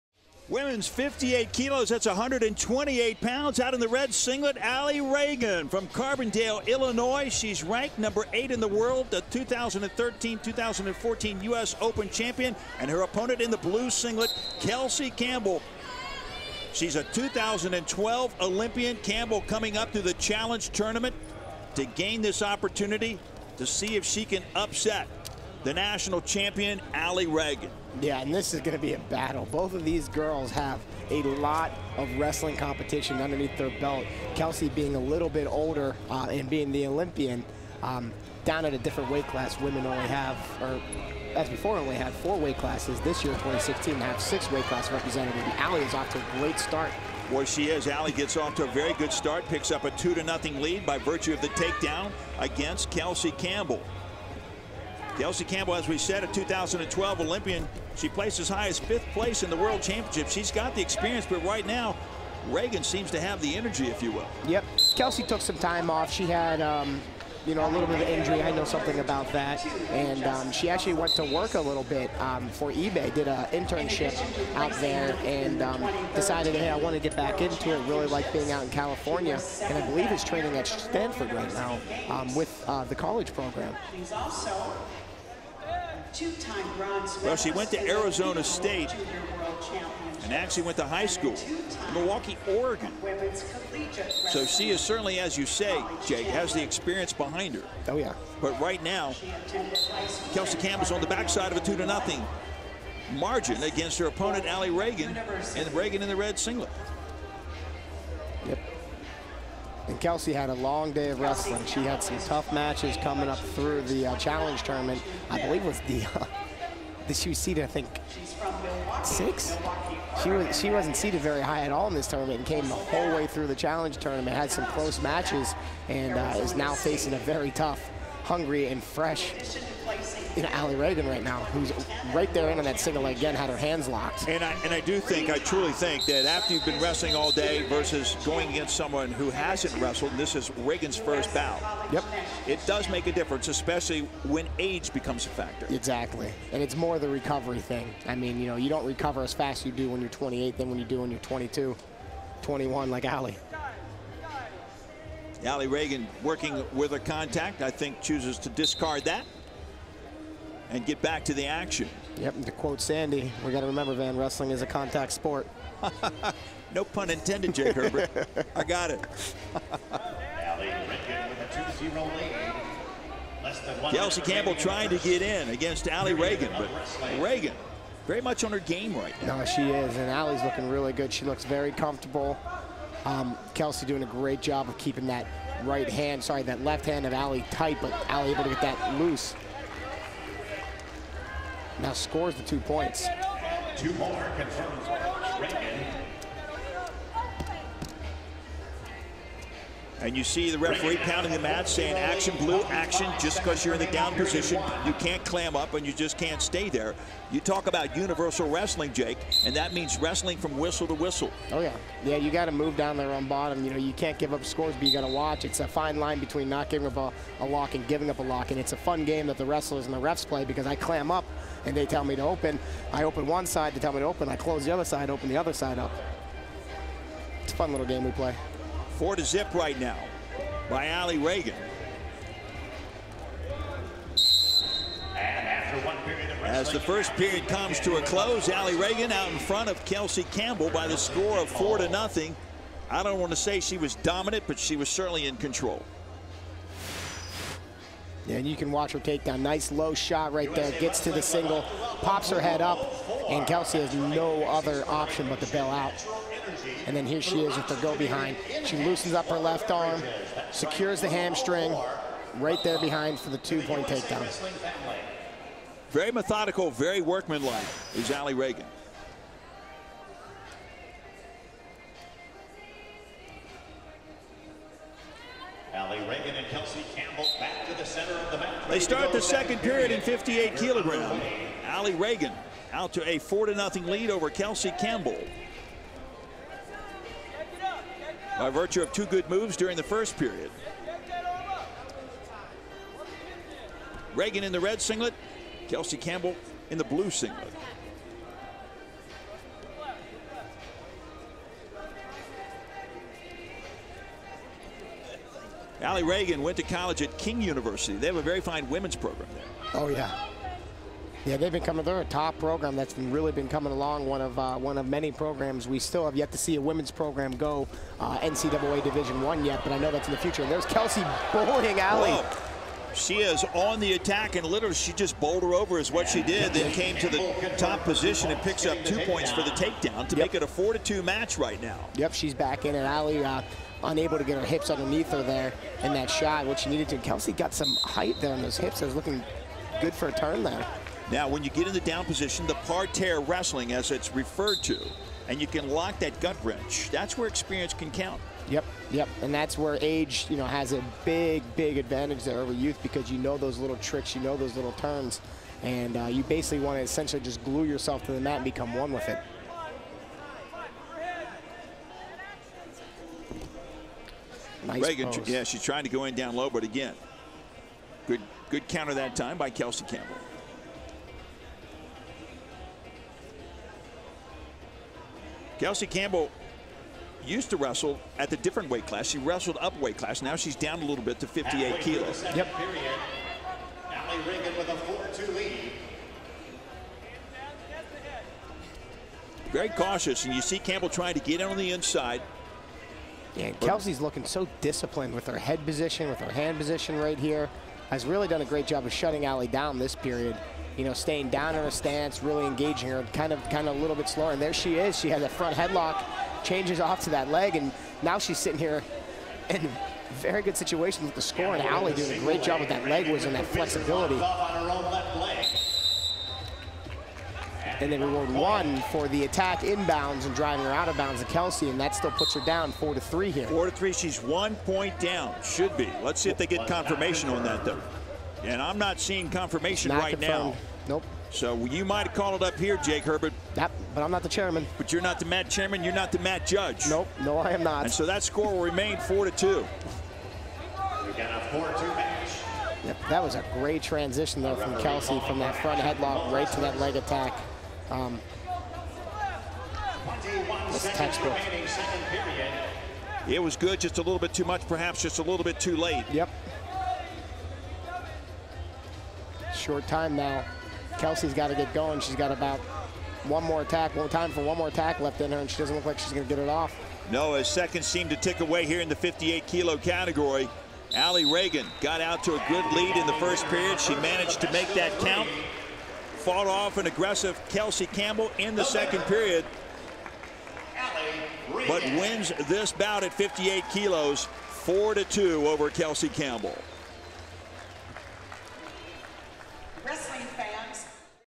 58kg Alli Ragan vs. Kelsey Campbell [1] 2014 Freestyle Wrestling World Team Trials
58 kg match between Alli Ragan  and Kelsey Campbell from the 2014 Freestyle Wrestling World Team Trials in Madison, WIsconsin.